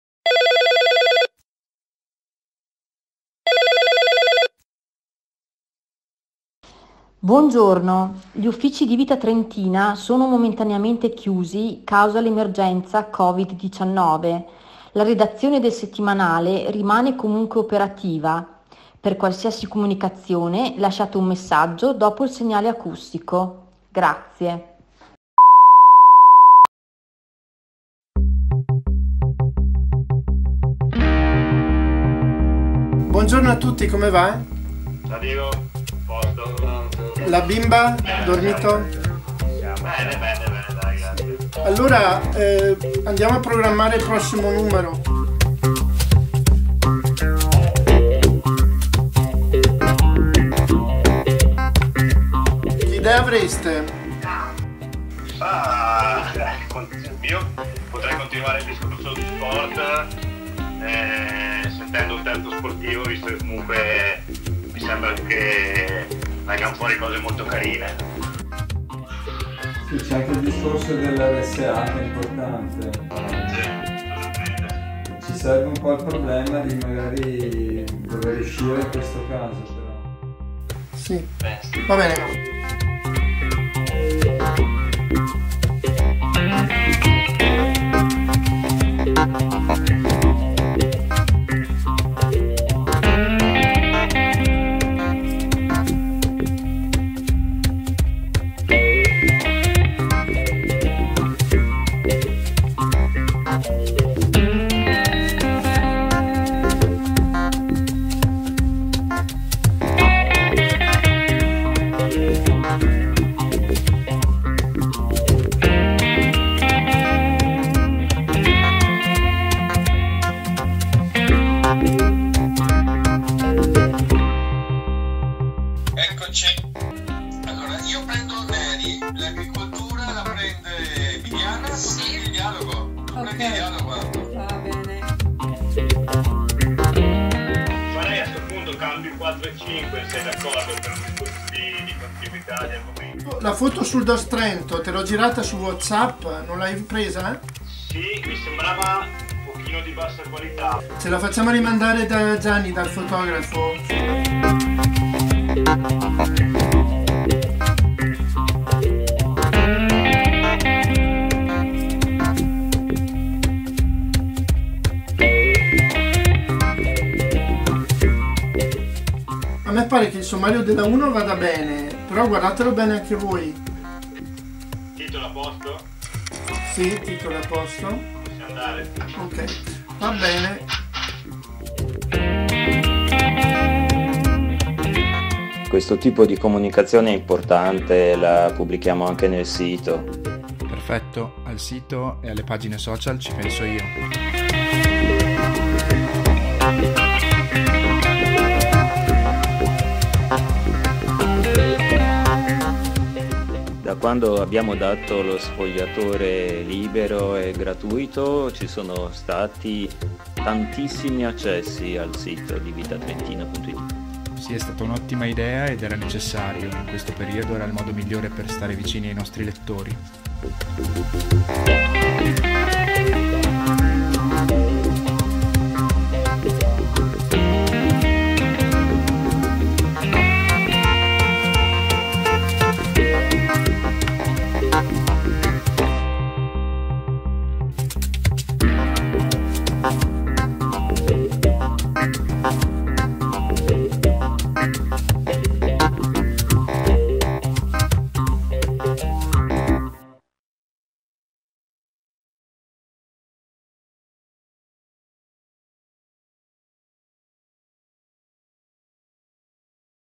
Vita trentina, così nasce il giornale al tempo del
Smart working a Vita Trentina per garantire l'uscita del settimanale. Ecco come nasce un nuovo numero, un'opera collettiva pensata e confezionata a distanza. Mentre al Vigilianum sulle scrivanie si deposita la polvere...